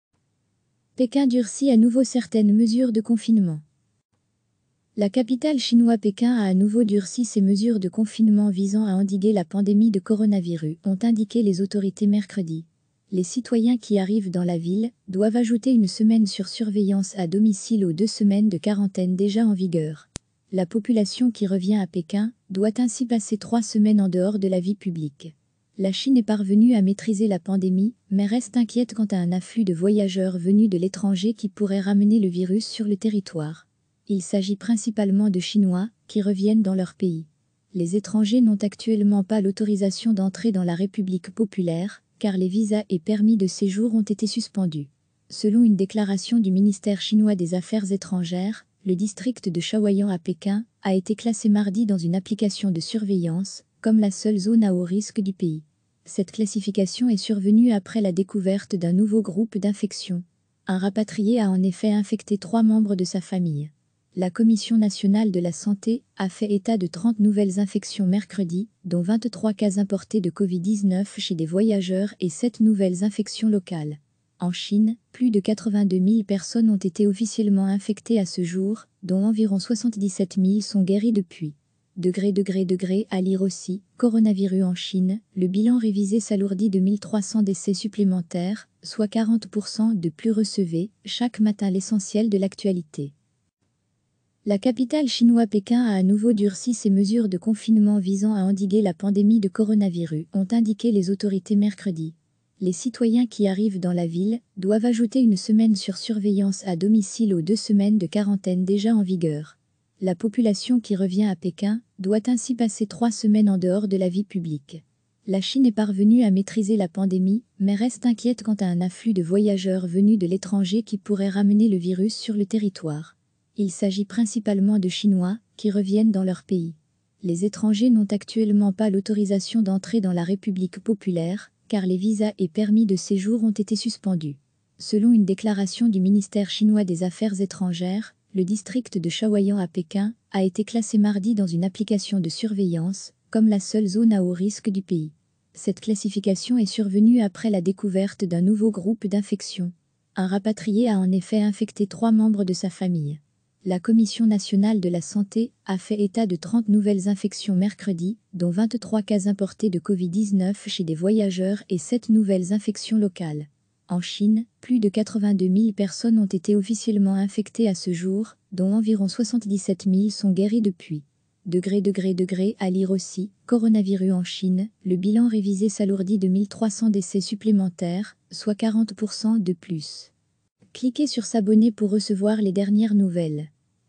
Pékin durcit à nouveau certaines mesures de confinement
La capitale chinoise Pékin a à nouveau durci ses mesures de confinement visant à endiguer la pandémie de coronavirus, ont indiqué les autorités mercredi. Les citoyens qui arrivent dans la ville doivent ajouter une semaine sur surveillance à domicile aux deux semaines de quarantaine déjà en vigueur. La population qui revient à Pékin doit ainsi passer trois semaines en dehors de la vie publique.

La Chine est parvenue à maîtriser la pandémie, mais reste inquiète quant à un afflux de voyageurs venus de l'étranger qui pourraient ramener le virus sur le territoire. Il s'agit principalement de Chinois qui reviennent dans leur pays. Les étrangers n'ont actuellement pas l'autorisation d'entrer dans la République populaire, car les visas et permis de séjour ont été suspendus.

Selon une déclaration du ministère chinois des Affaires étrangères, le district de Chaoyang à Pékin a été classé mardi dans une application de surveillance comme la seule "zone à haut risque" du pays. Cette classification est survenue après la découverte d'un nouveau groupe d'infection. Un rapatrié a en effet infecté trois membres de sa famille.

La Commission nationale de la santé a fait état de 30 nouvelles infections mercredi, dont 23 cas "importés" de Covid-19 chez des voyageurs et sept nouvelles infections locales. En Chine, plus de 82.000 personnes ont été officiellement infectées à ce jour, dont environ 77.000 sont guéries depuis.

►►► À lire aussi : Coronavirus en Chine : le bilan révisé s'alourdit de 1300 décès supplémentaires, soit 40% de plus